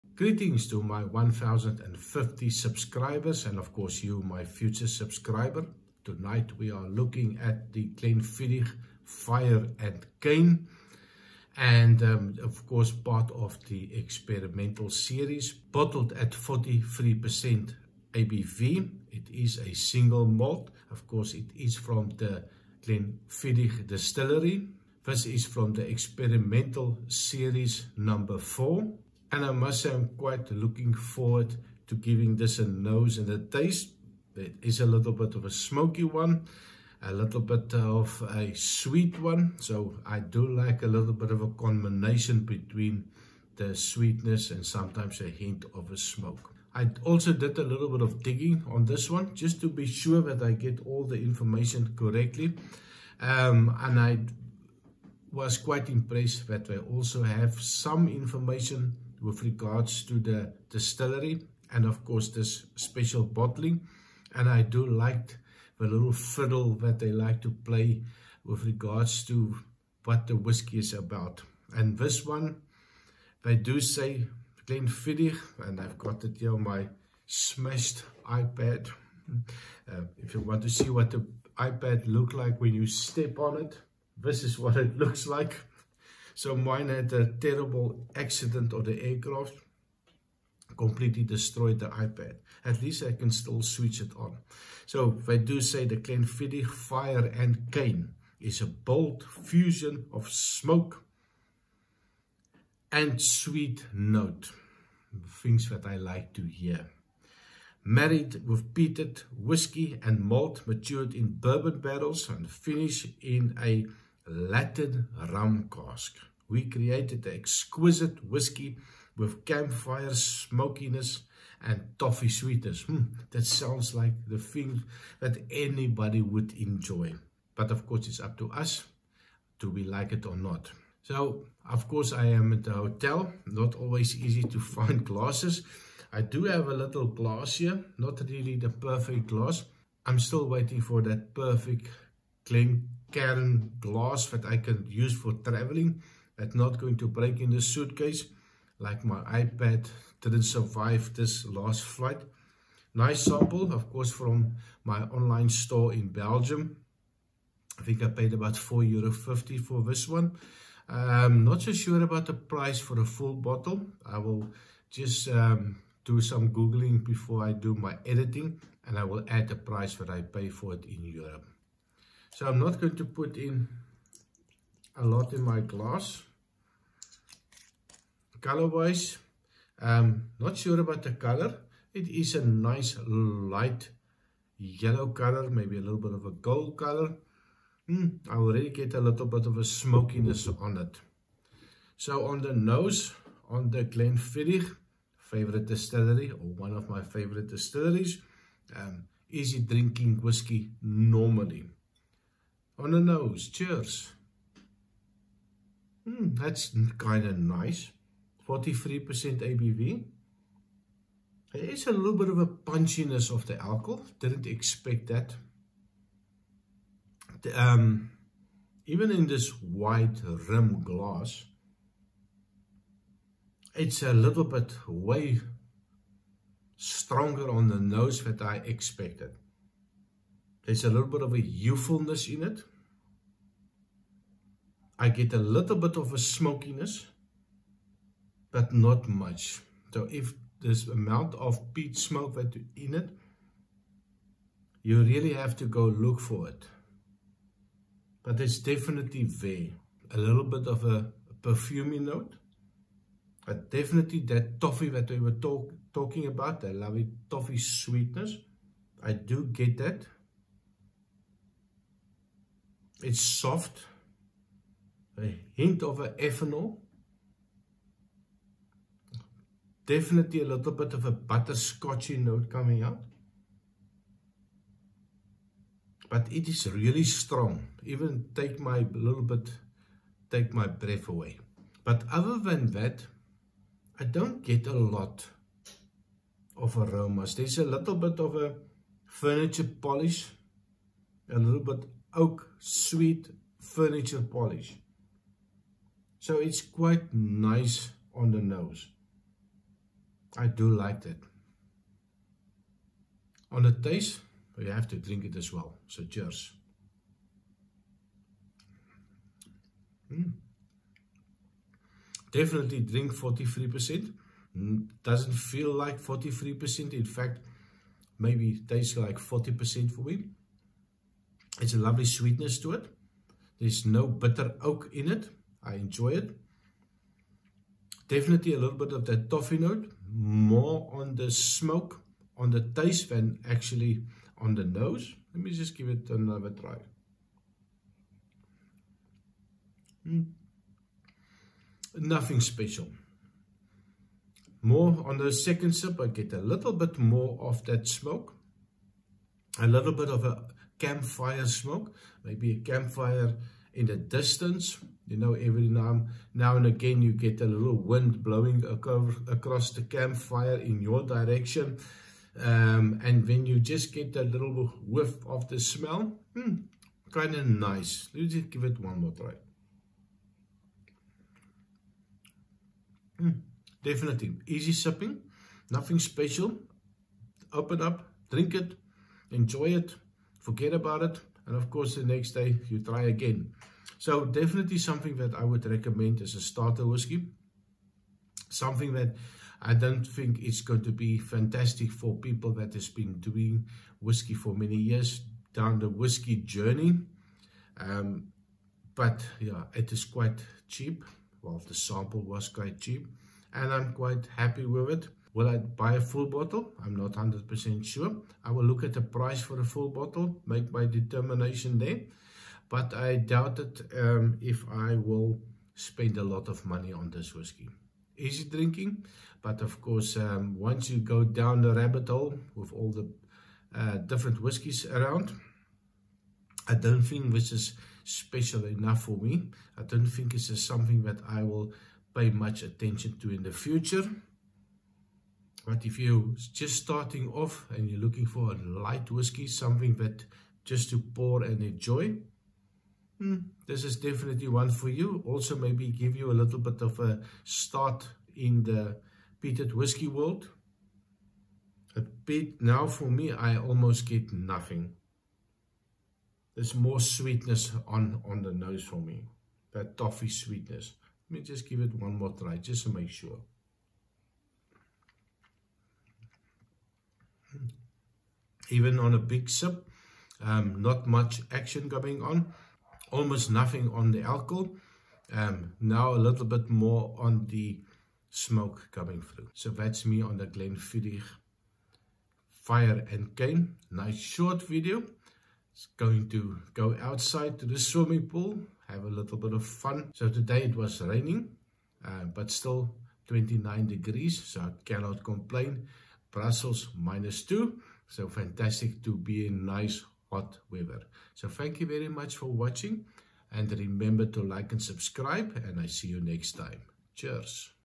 Glenfiddich Fire and Cane | Whisky review
Another release from Glenfiddich.  This is the Glenfiddich fire and cane experimental series 4.